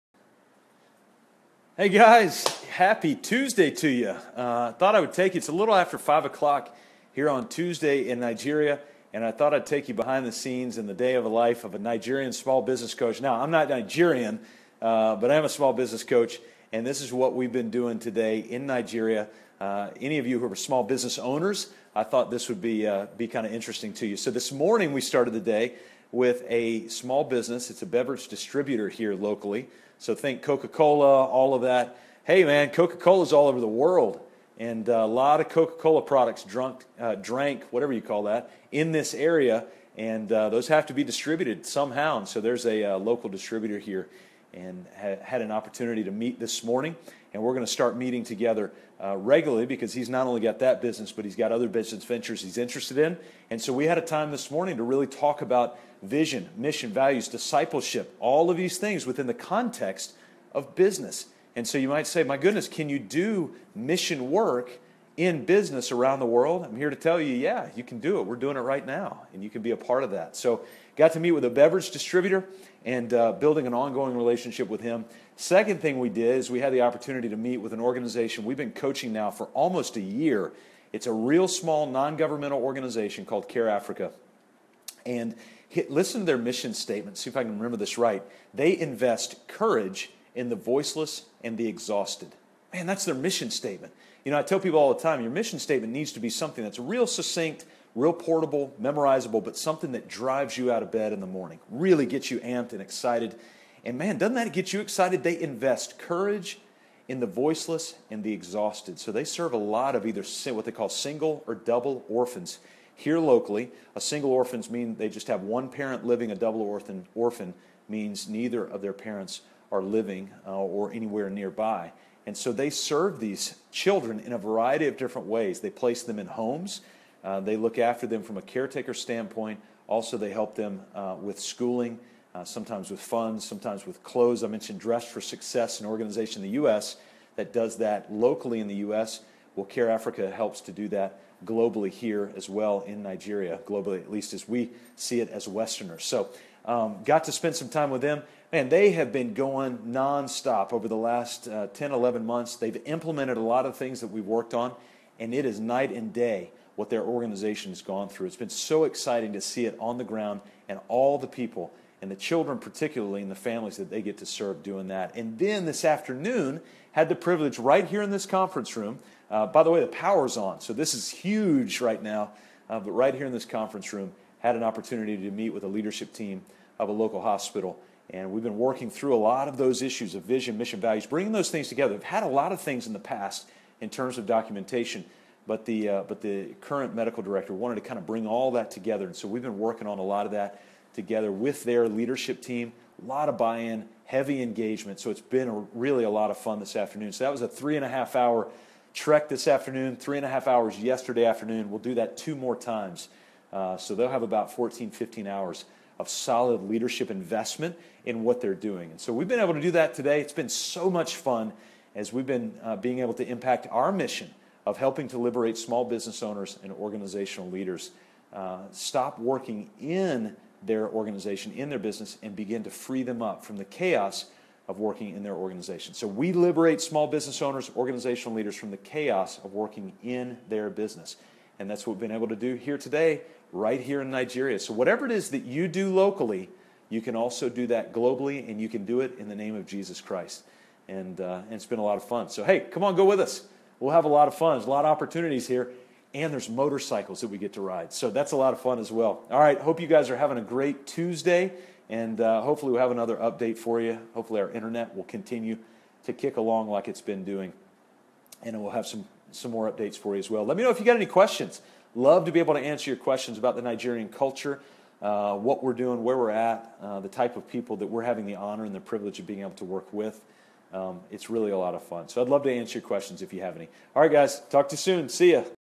Day in the life of a Nigerian Small Business Coach: Beverage Distributor, Non-Governmental, and
Day in the life of a Nigerian Small Business Coach: Beverage Distributor, Non-Governmental, and a Hospital Management team...